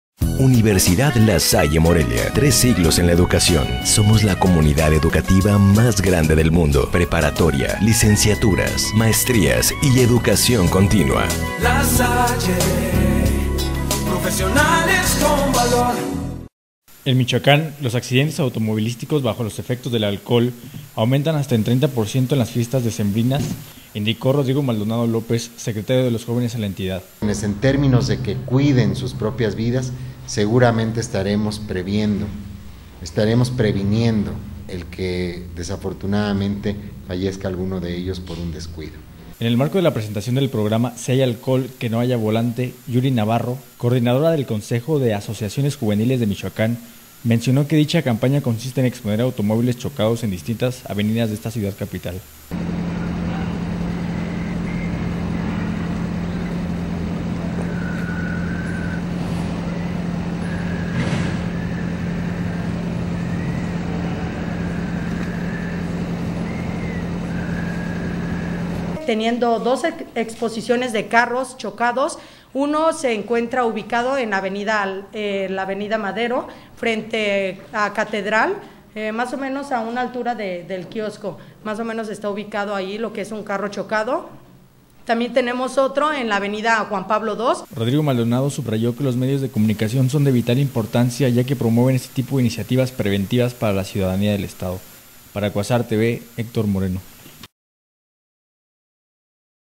Arranca en Morelia “Si hay alcohol que no haya volante”
EN  Michoacán, los accidentes automovilísticos bajo los efectos del alcohol aumentan hasta en un 30 por ciento en las fiestas decembrinas, indicó Rodrigo Maldonado López, secretario de los Jóvenes en la entidad…